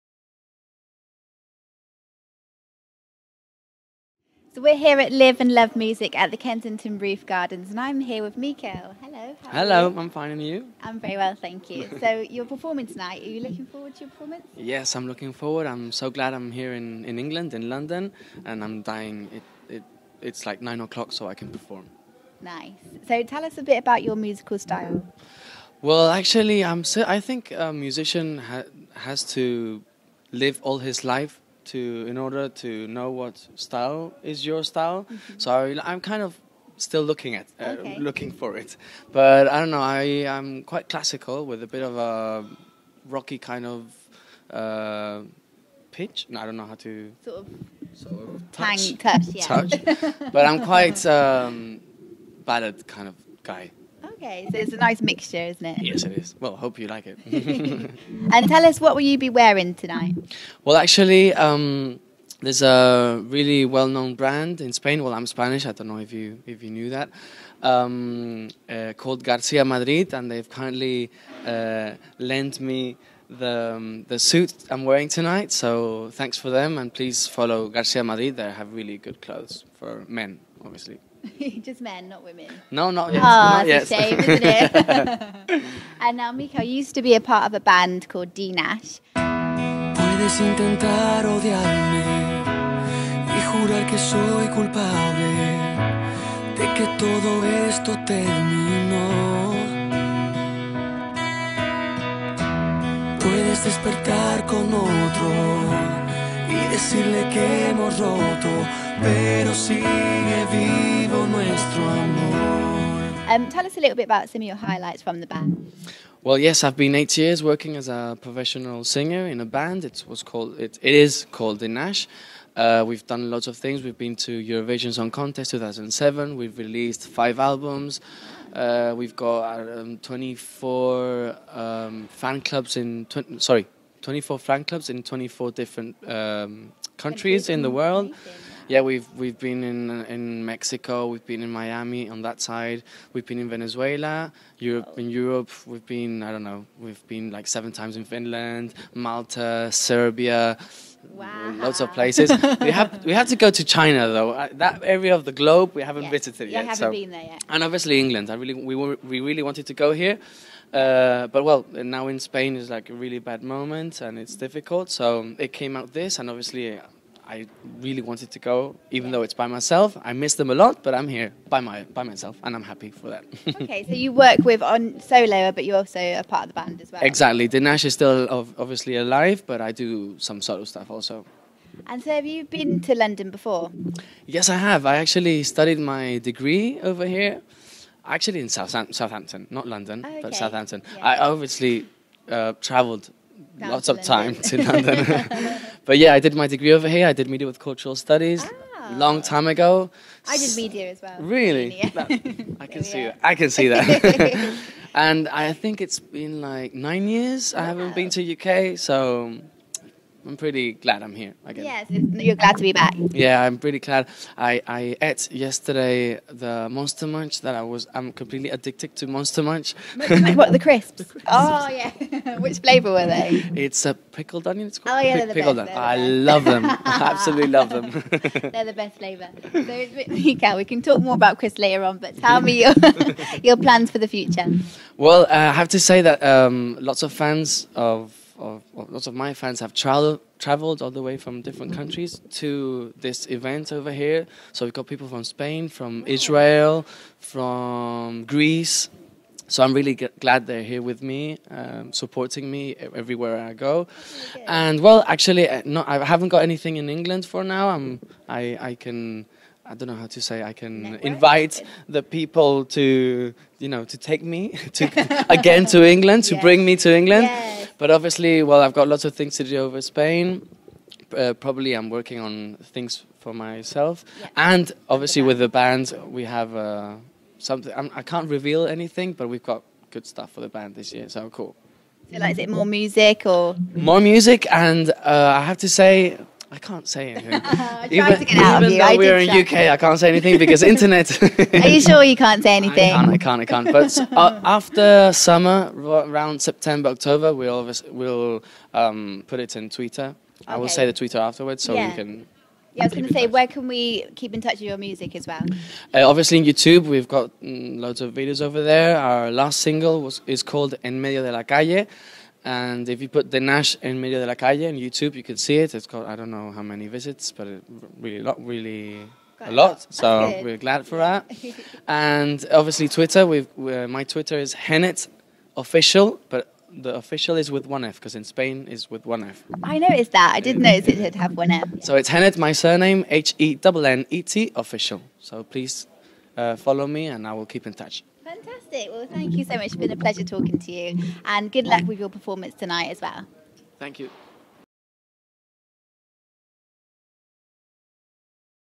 Mikel Hennet (D'Nash) Interview with Bianca Clayton | Live & Love Music
Spanish singer Mikel Hennet (from D'Nash) is invited to perform in London for Live & Love Music's 5th Birthday bash at Kensington Roof Gardens and is interviewed before going on stage with Bianca Clayton.